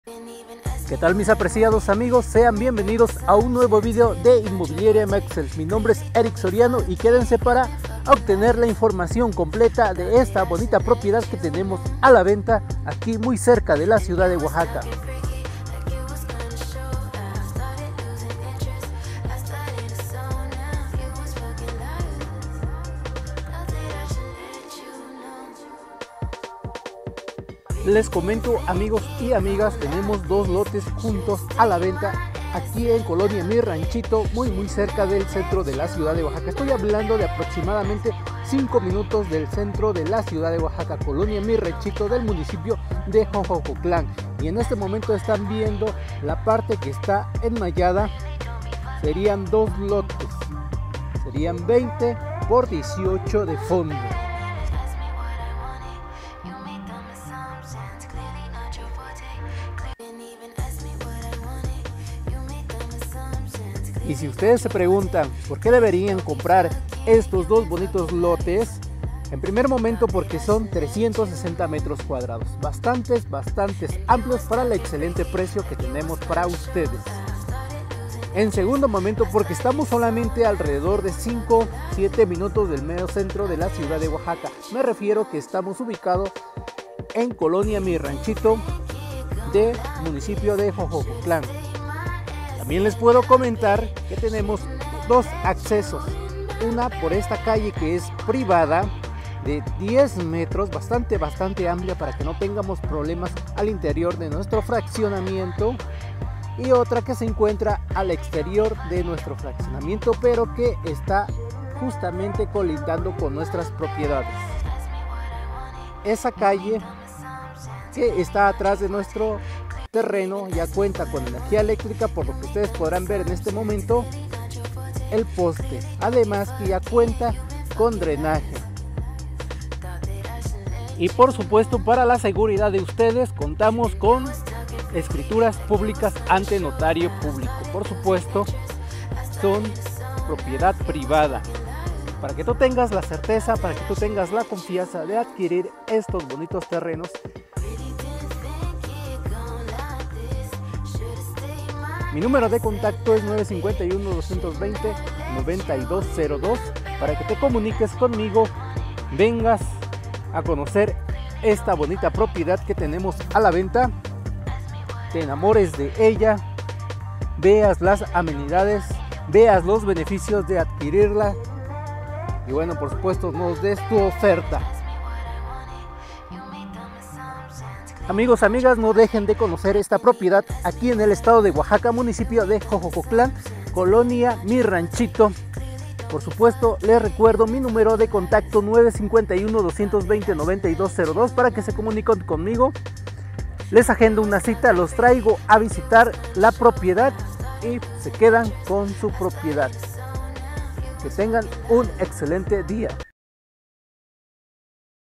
Terreno en venta. Xoxocotlan, Oaxaca
Se venden 2 lotes juntos, a 1 cuadra de la avenida en xoxocotlan, colonia Mi Ranchito a 7 min de la ciudad de oaxaca. En total son 360 m2. Su precio es de $890,000. A tratar